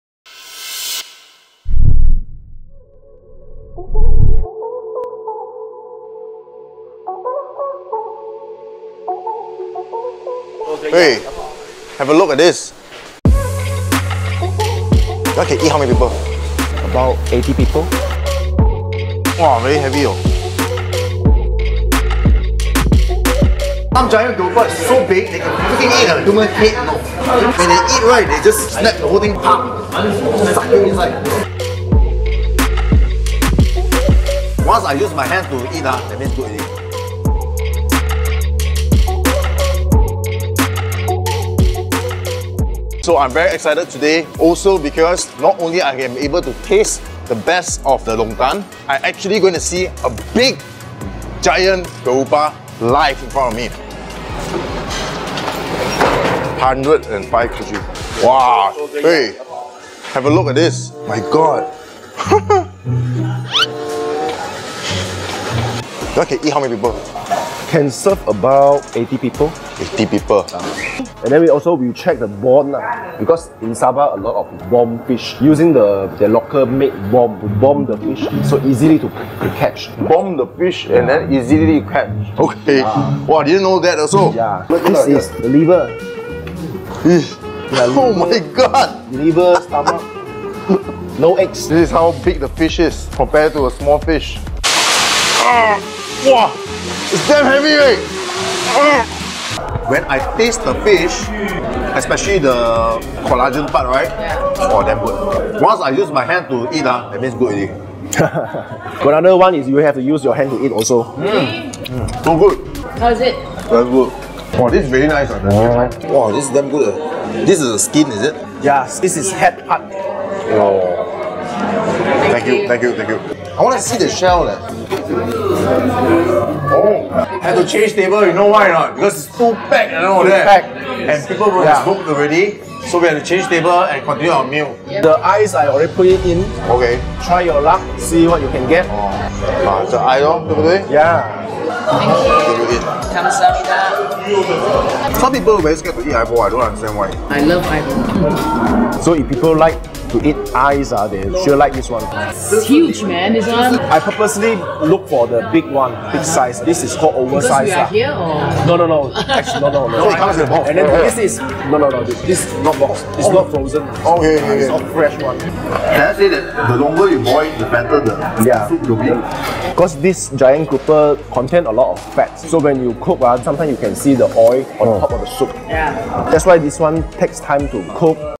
SZN 1 | EPI 1 | 1ST TRAILER - Ah Lau Food King (Dennis Lau) Eats 105KG Giant Grouper Fish @ KK SABAH
Location : Sri Maju Steamboat 福华火锅
Address: Lot G34, Kompleks Phase 2A Jalan Asia City Pusat Bandar Kota Kinabalu 88000 Kota Kinabalu Sabah, Malaysia